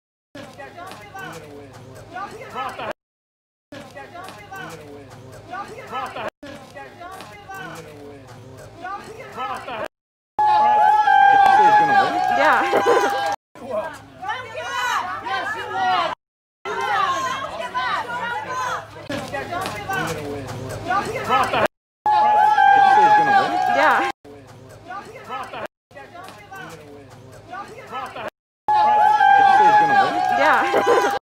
"We're Gonna Win, Wait" - Donald Trump At Golf Course On Day Biden Declared Winner By Media.
"We're Gonna Win, Wait" or "We're Gonna Win, Watch"

What do you hear?